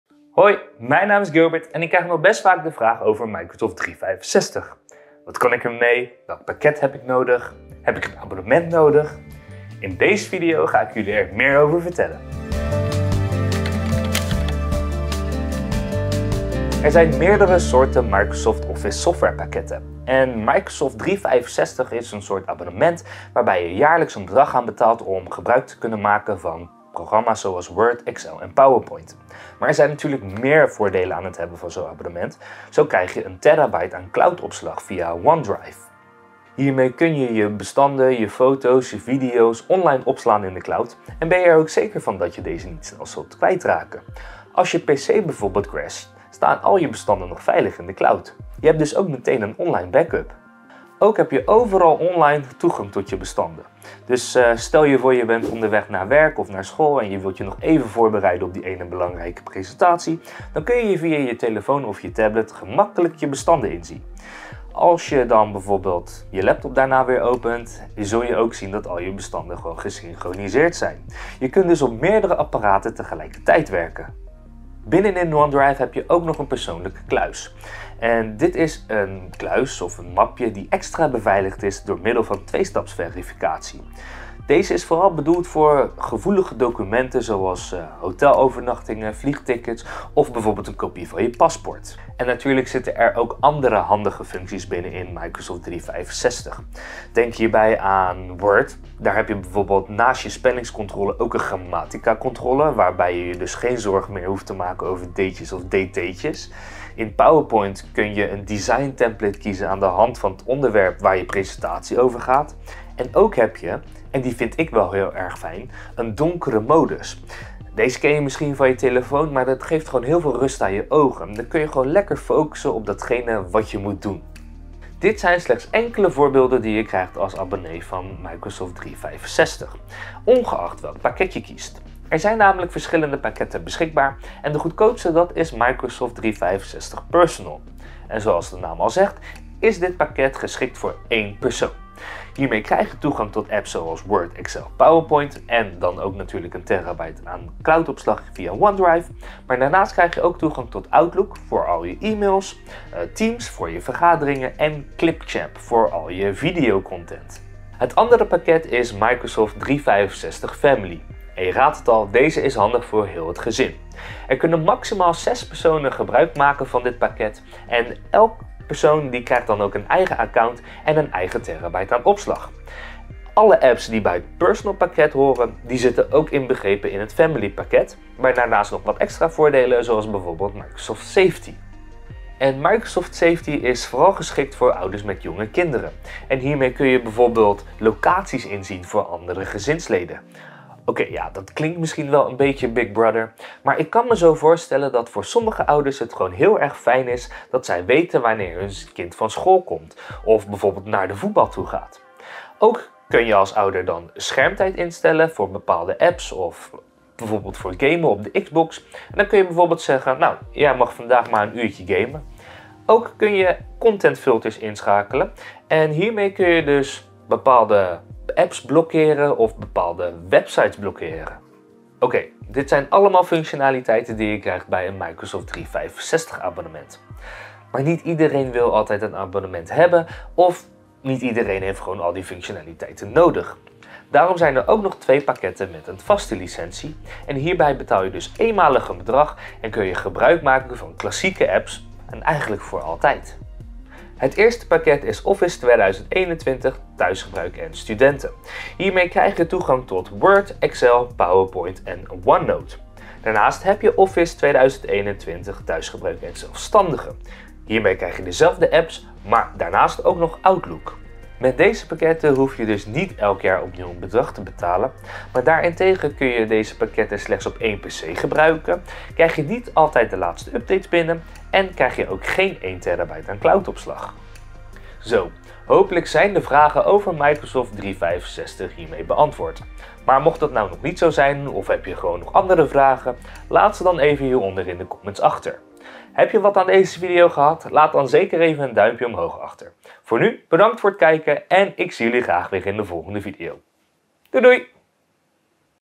Microsoft 365 - Wat kan ik er mee?
Er zijn verschillende Microsoft Office software pakketten: Microsoft 365 Personal en Family en Microsoft Office 2021. In deze video legt Gilbert je uit wat de verschillen zijn en welk pakket het beste bij je past.

0:00 Intro
0:17 Microsoft 365
0:30 OneDrive
1:32 Extra functies Microsoft 365
2:12 Microsoft 365 Personal
2:38 Microsoft 365 Family
3:02 Microsoft Safety
3:42 Office 2021
4:21 Verschillen Microsoft 365 vs Office 2021